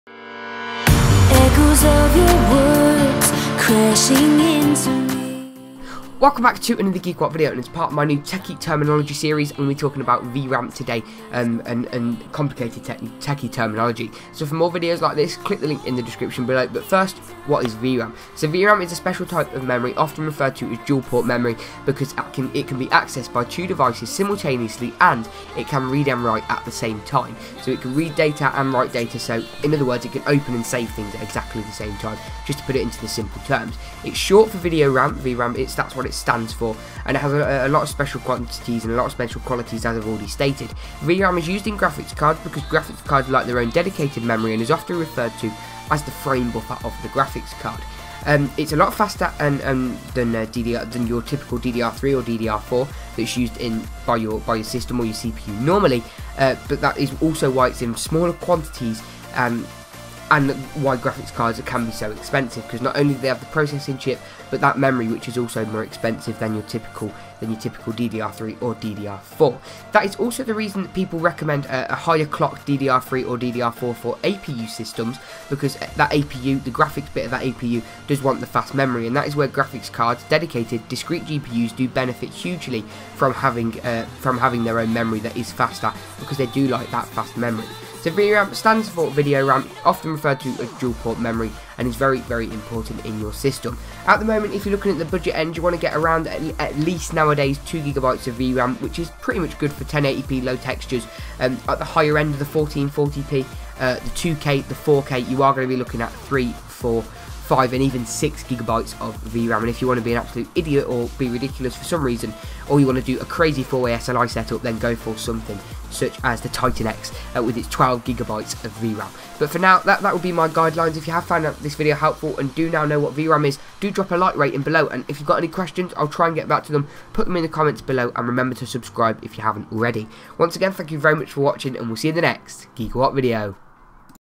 What is VRAM?! [ASKED & ANSWERED!]
Wondered how VRAM is used, what it's used for and why it's needed?! If so you're in the right place all you need to nw & more!

GeekaWhat - PC Builds, in the US and UK currencies (£-GBP + $-USD) from $200 (£150) to fully fledged setups of 16K!